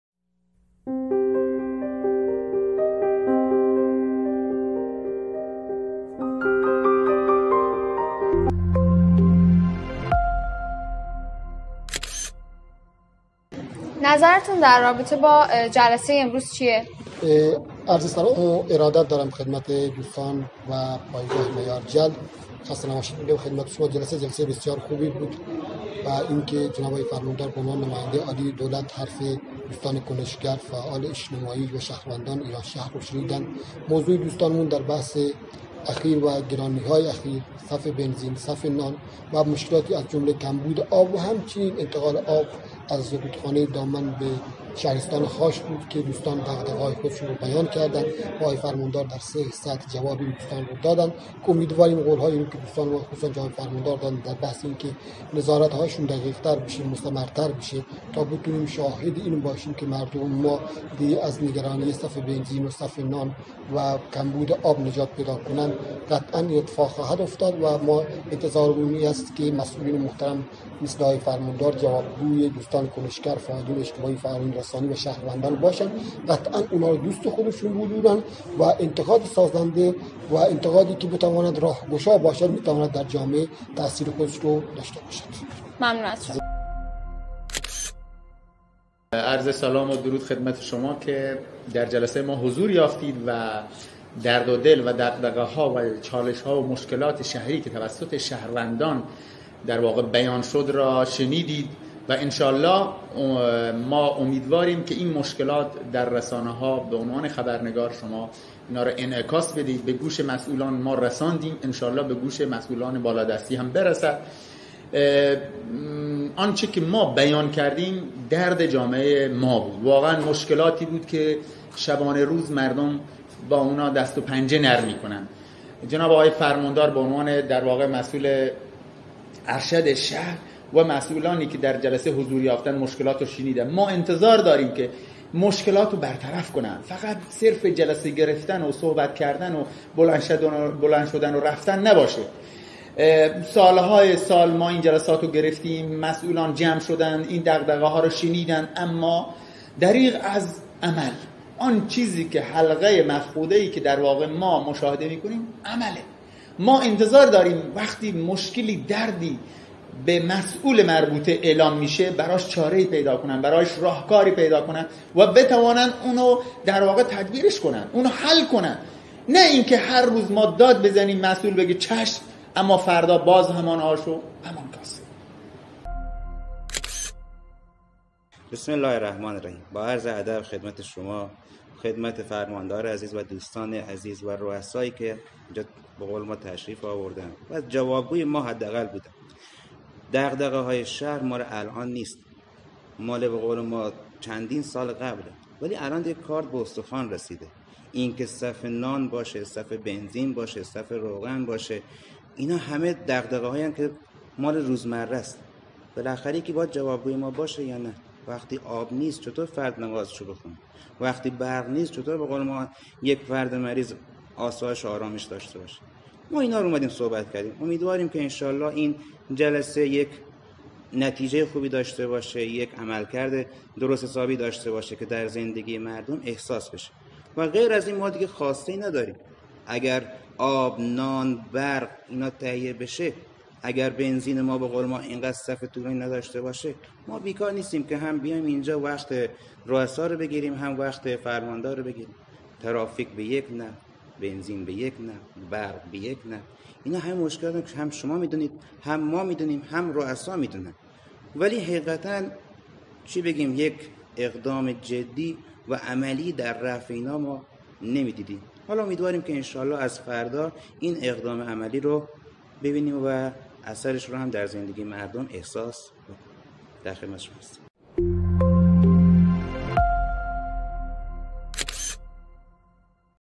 نشست فعالین اجتماعی با چاشنی مطالبه‌گری در دیدار با فرماندار و مسئولان ایرانشهر
🔻شهرستان ایرانشهر همچون دیگر شهرهای بلوچستان با مشکلات زیادی نظیر صف های طولانی بنزین و نان و دیگر معضلات گریبانگیر است؛ جهت چاره اندیشی و رفع این مشکلات جلسه ای بین فرماندار شهرستان و مسئولان ادارات و فعالان اجتماعی برگزار شد و به مشکلات ایرانشهر پرداخته شد

✅ میارجل ، رسانه فعالان فرهنگی اجتماعی اقتصادی بلوچستان